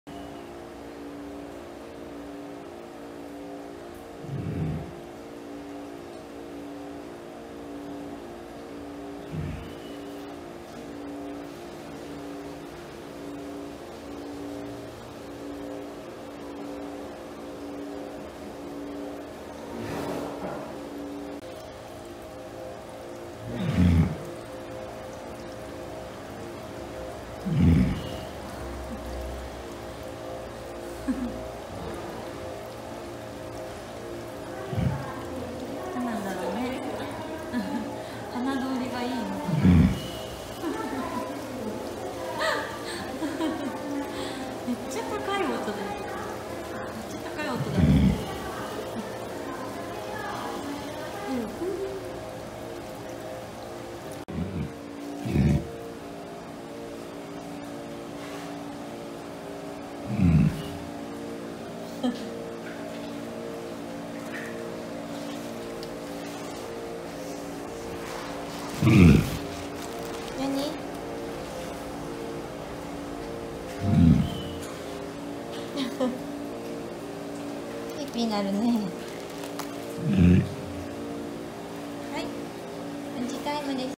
【セイウチ】タンポポの鼻ピーピー動画（伊勢シーパラダイス）
セイウチのタンポポちゃん(^-^;
たまーーに「鼻がピーピーする日」があるんです（笑）